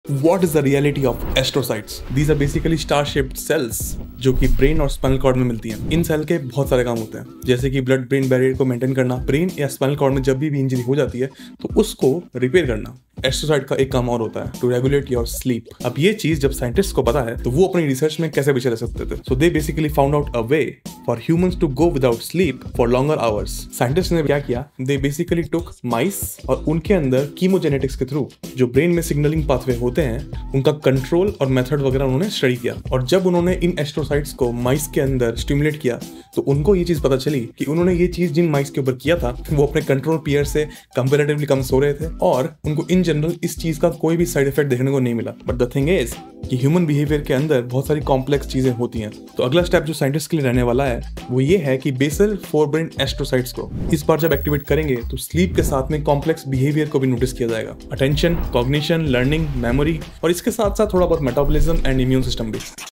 The mice who never slept| Dr.Nitin Khajotia | Science
You can't have more than 24 hours in a day but you can always have more time by not sleeping and that's where this research comes in
Research Finds Little-Studied Brain Cells Could Be The Reason Of Staying Awake Without Cost To Cognition, Health

Psylogic - Life / Science based Topics
My Gear
Apple Pencil